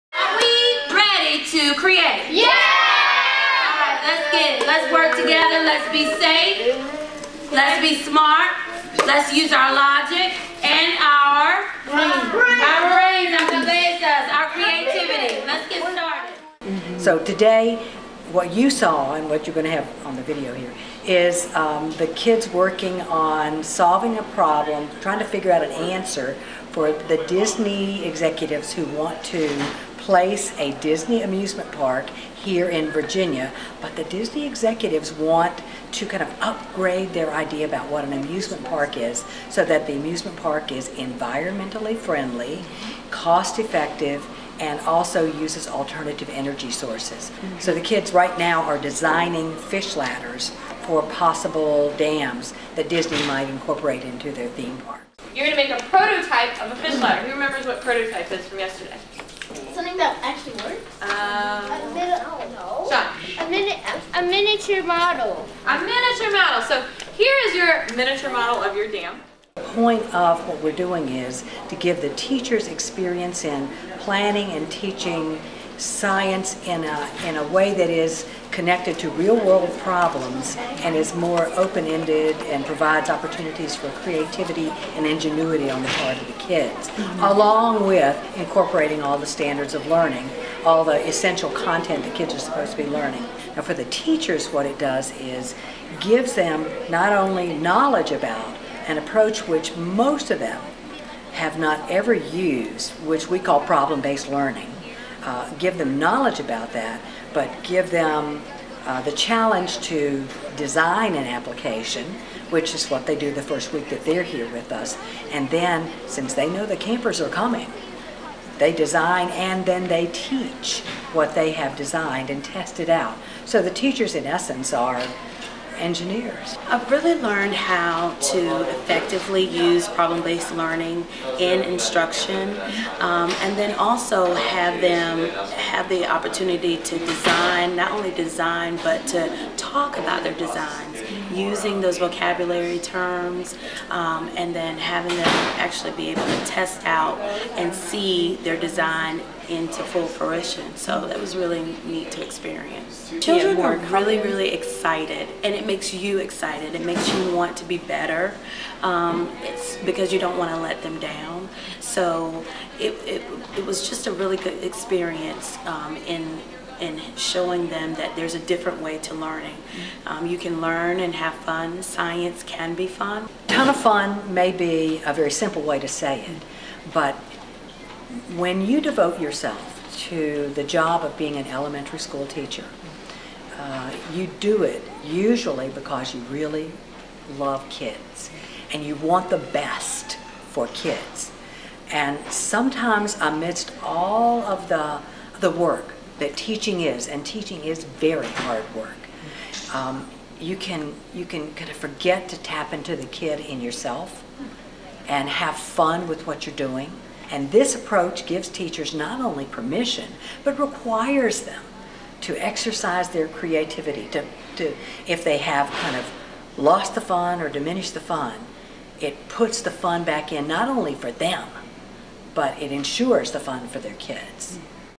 VISTA: A problem-based learning lab
The Virginia Initiative for Science Teaching and Achievement (VISTA), a program teaching teachers how to incorporate problem-based, hands-on learning into their science lessons, we held this summer at the William & Mary School of Education with more than 40 area teachers attending.

Funded by a five-year, $34 million grant from the U.S. Department of Education, VISTA is a partnership among more than 60 Virginia school districts, the Virginia Department of Education and six Virginia universities, including lead institution George Mason University.